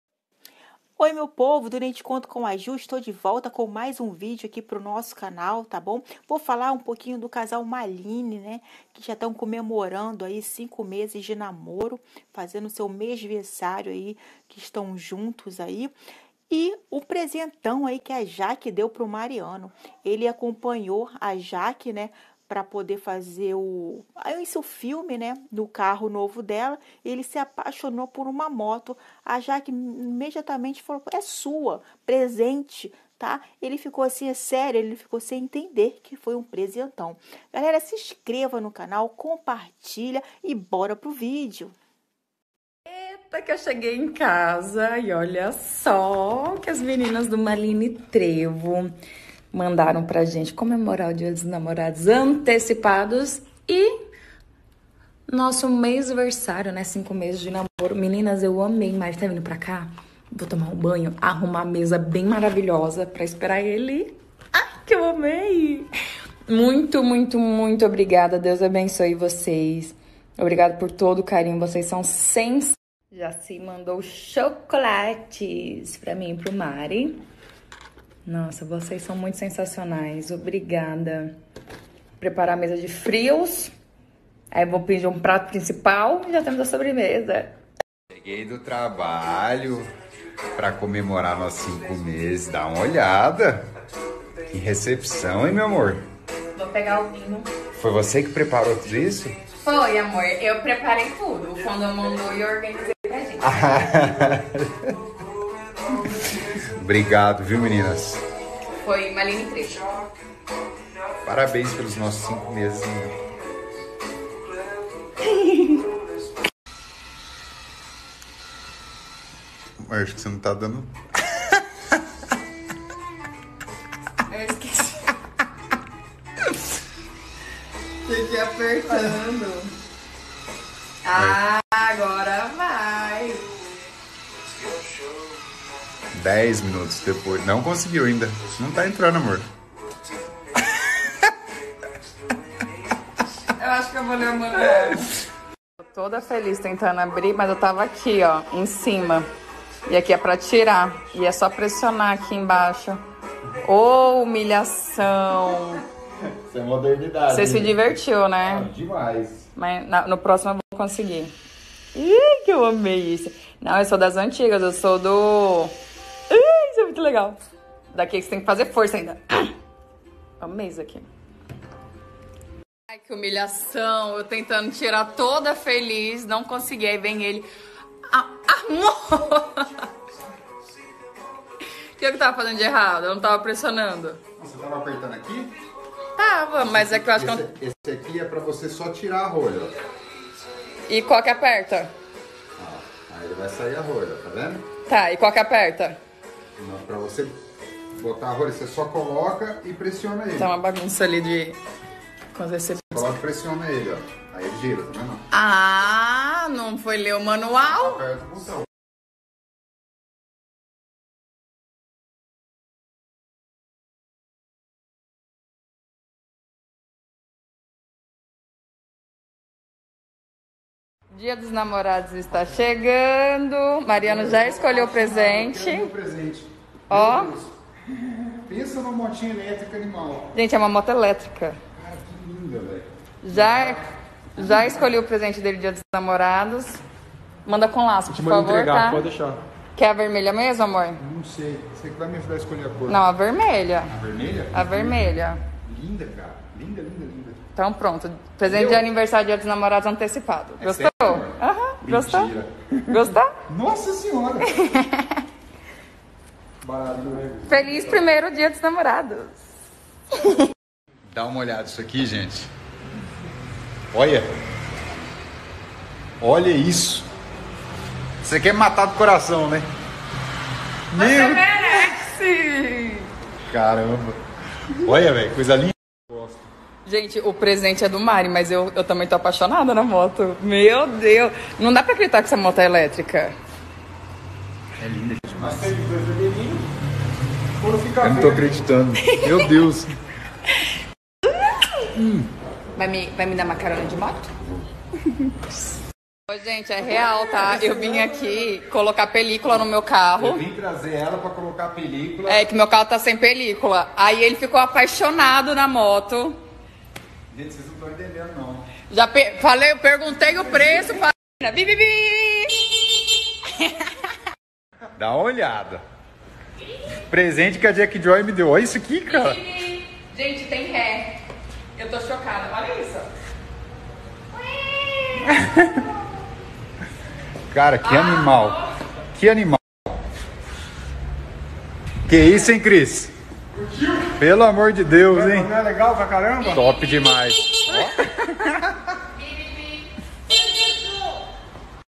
👸CASAL MALYNE- COMEMORAÇÃO DOS 5 MESES DE NAMORO / PRESENTÃO DA NAMORADA.
TODOS OS DIAS TEM ACHADINHOS 🥰
 O CASAL COMEMOROU 5 MESES DE NAMORO, COM NOITE DE QUEIJOS E VINHO PRESENTEADOS PELO FÃ CLUBE CASAL.
JAKE DEU UMA MOTO PARA MARIANO PRESENTE ANTECIPADO DE DIA DOS NAMORADOS.